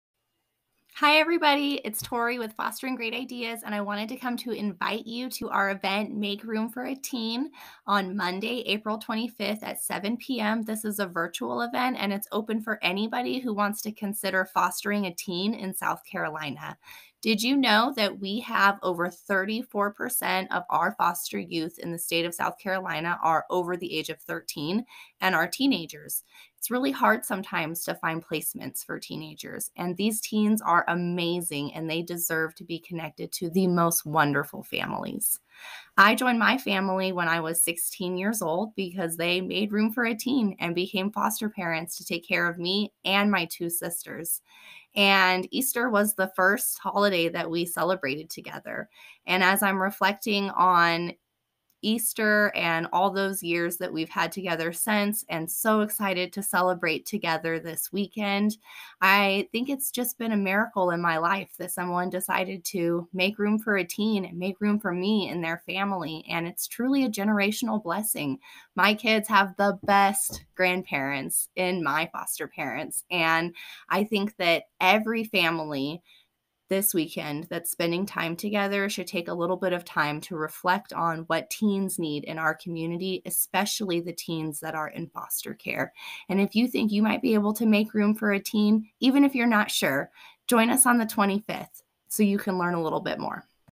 Fostering a Teen: a great video to begin with
Hear from Tori as she makes it all so real.